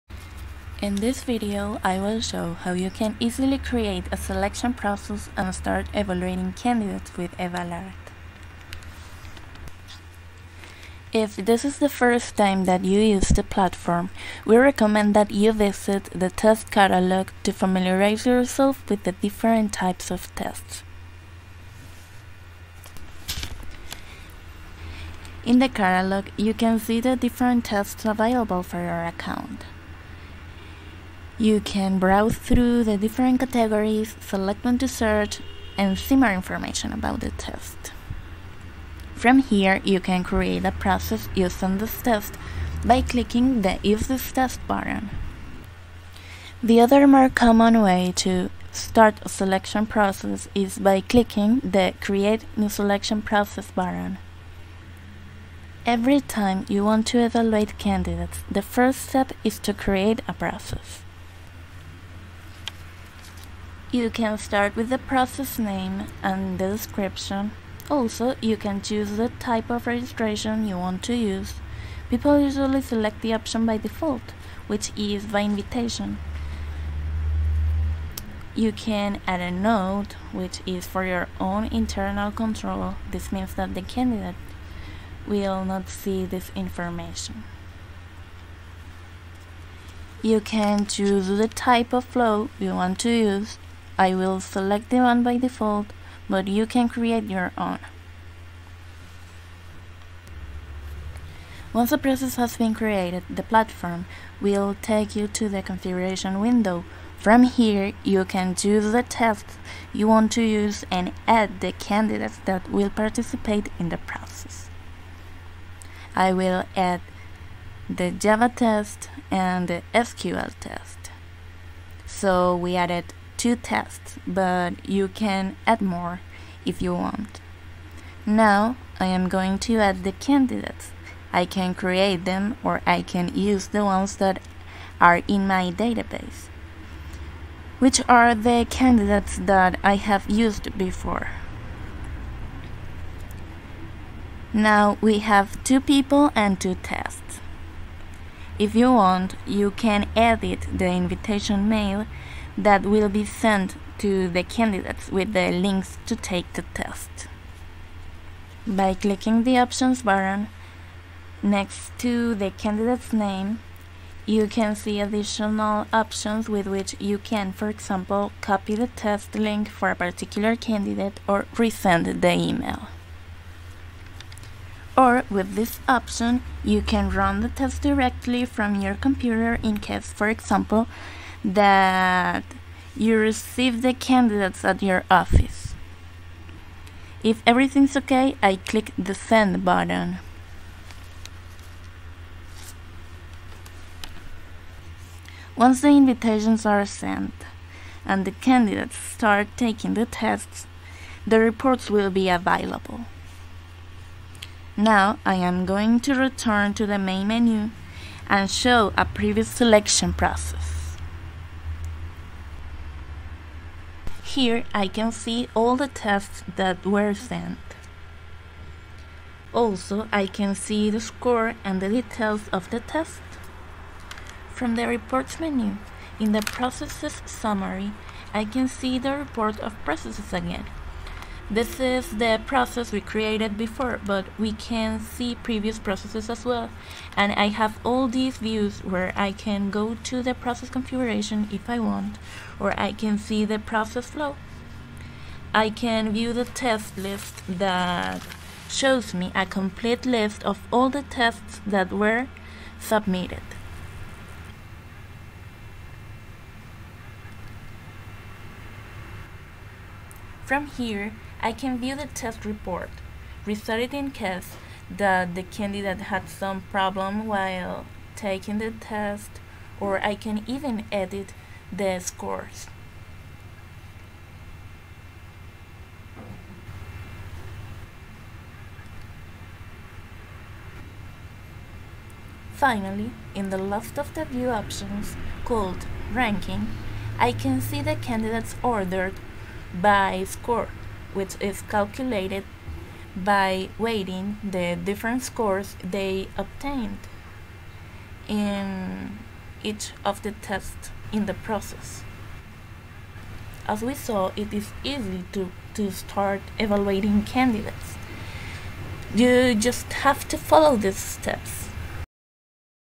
Evalart Explainer Video
This video shows how to start using Evalart. It explains how to create a recruiting process, add tests, add candidates and send them the tests. Then it show how to view the tests results.